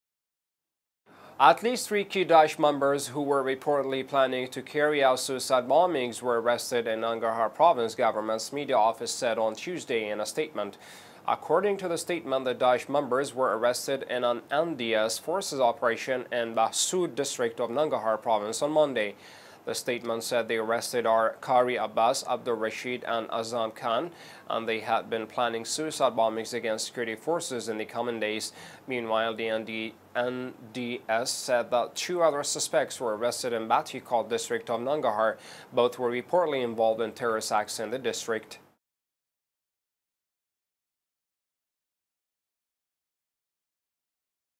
Daeshbomb plotters arrested in Nangarhar
At least three key Daesh members, who were reportedly planning to carry out suicide bombings, were arrested in Nangarhar province, government’s media office said on Tuesday in a statement.
According to the statement the Daesh members were arrested in an NDS forces operation in Behsod district of Nangarhar province on Monday.
The statement said the arrested are Qari Abbas, Abdul Rashid and Azam Khan, and they had been planning suicide bombings against security forces in the coming days.
Meanwhile, the NDS said that two other suspects were arrested in BatiKot district of Nangarhar. Both were reportedly involved in terrorist acts in the district.